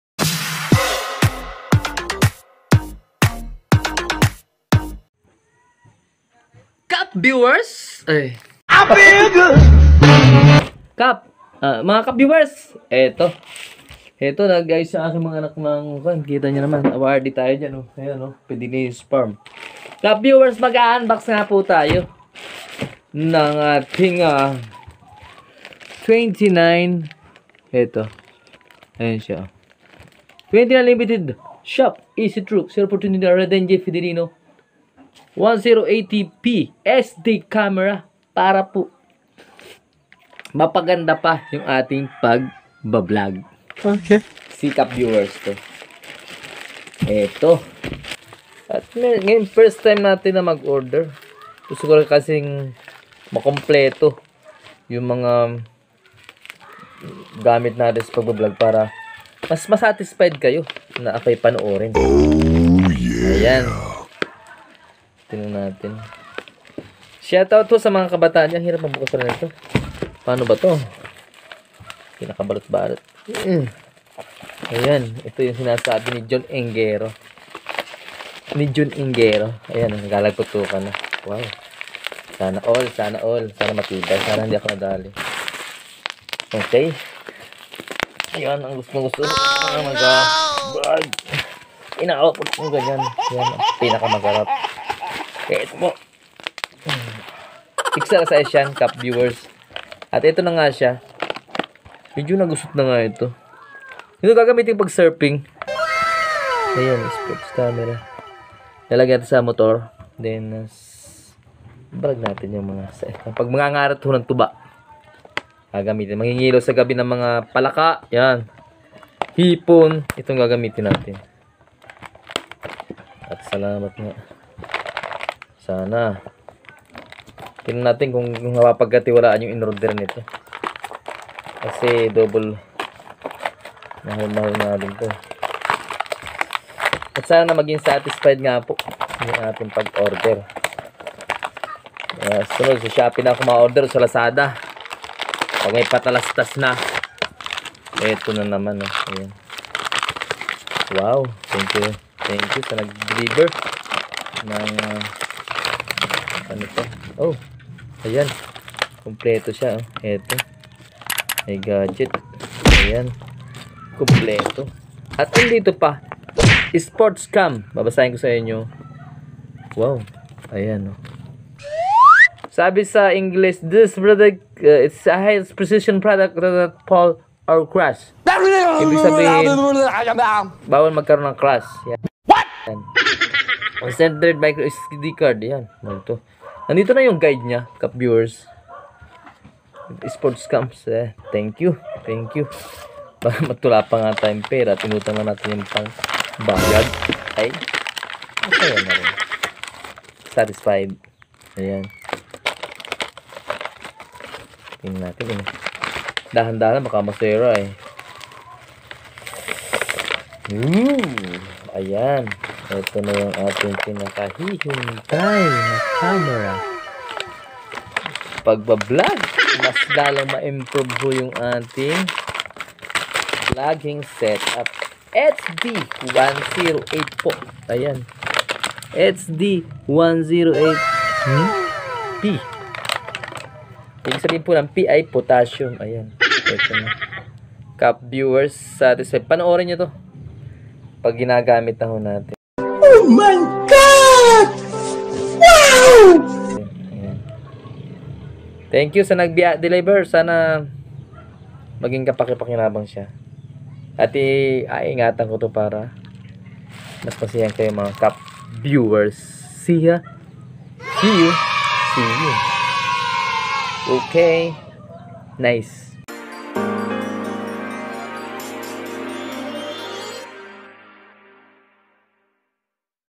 UnBOXING HD1080 CAMERA
ITo na mga Kap VIEWERS..ang bago nating kagamitan sa pag ba-VLOG..para mas Lalo pang mpganda Ang ating Mga Video na Inaupload..
Let's SEE Kap VIEWERS..😍🙄🤔